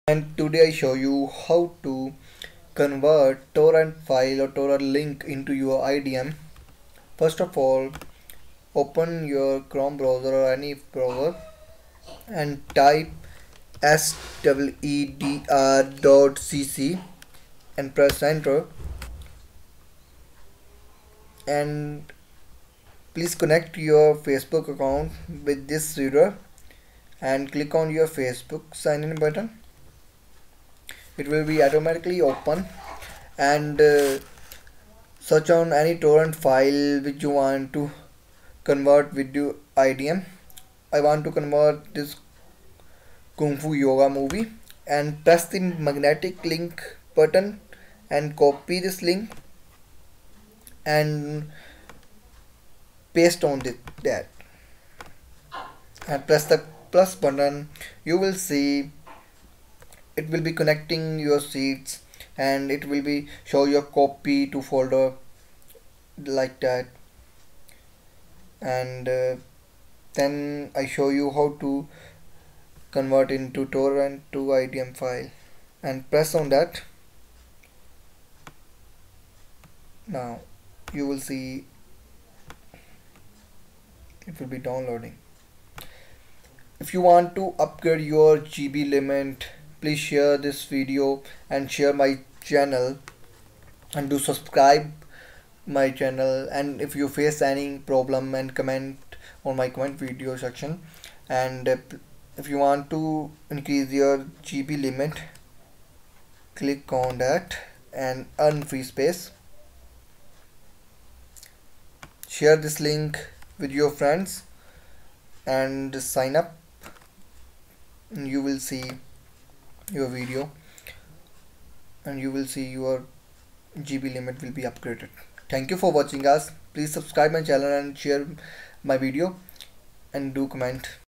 How To Convert Torrent File Into IDM
Hi Friends Today I show you
How to convert your Torrent file into your IDM file
It is so simple please follow my step
And sign up with your Facebook account then open your Torrent website and copy your magnetic link and paste on the seedr website.